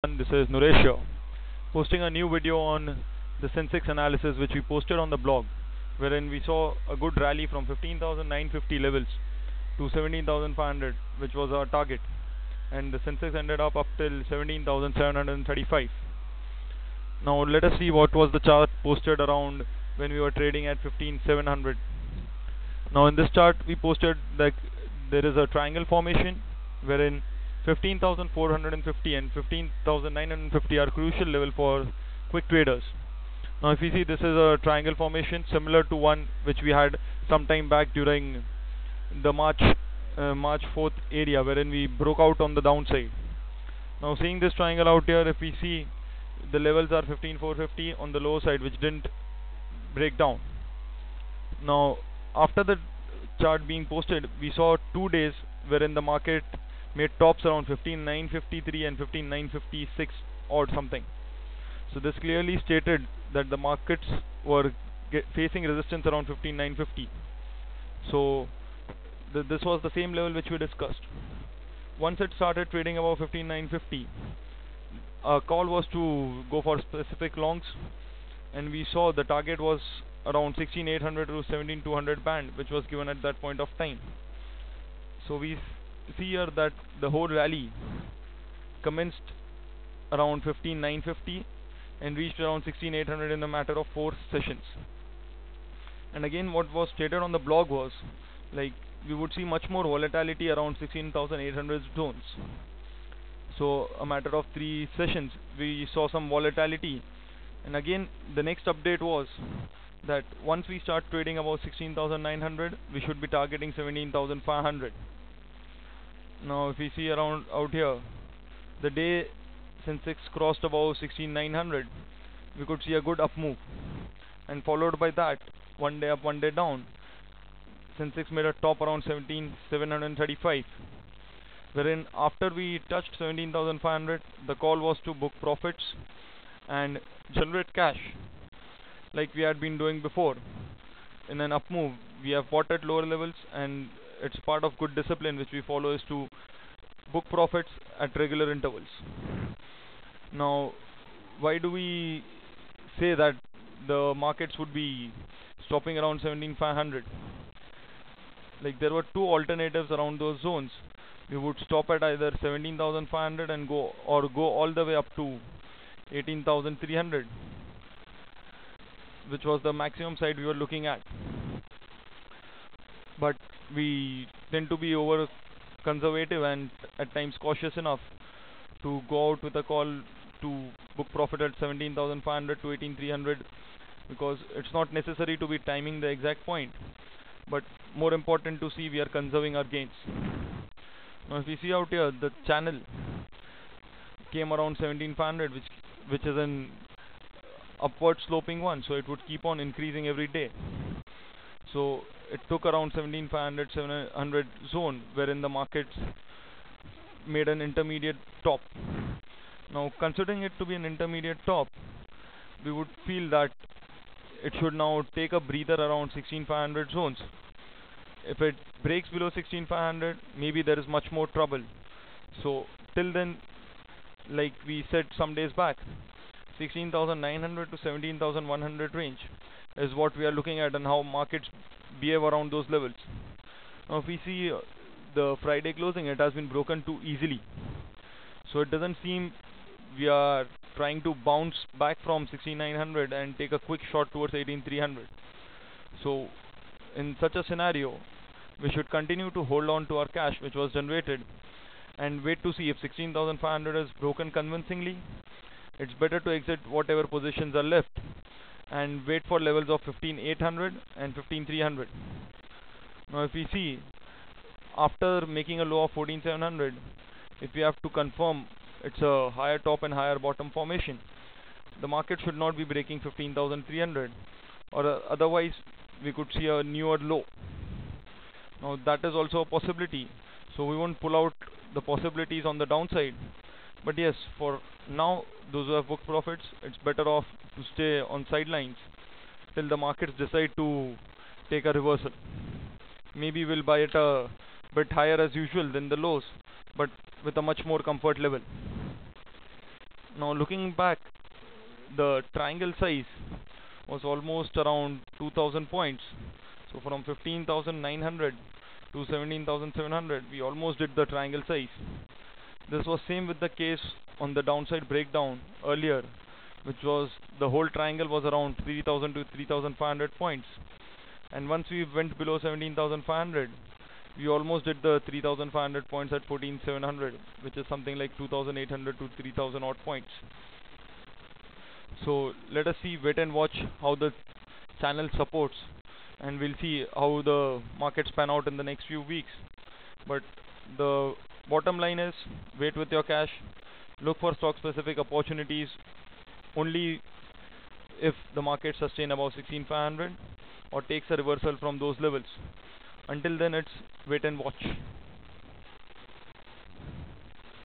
SENSEX ANALYSIS
Analysis of the Sensex run from 15950 to 17735 explained.